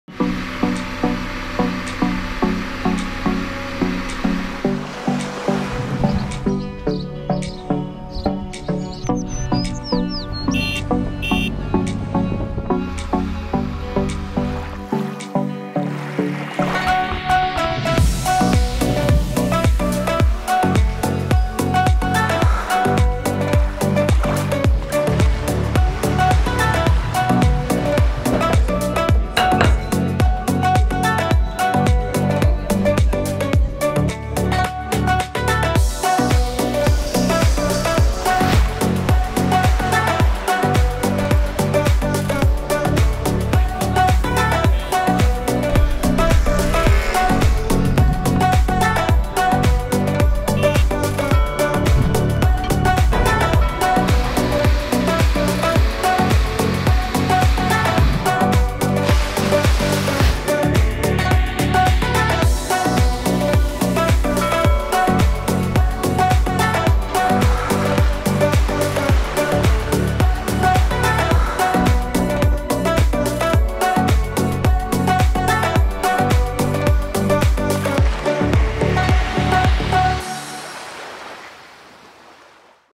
Thailand inspiratie | door medewerkers van Shoestring
Onze medewerkers hebben allemaal een passie voor reizen en gaan regelmatig op reis. Zo ging collega Veronique laatst naar Thailand waar zij haar avonturen heeft vastgelegd op film.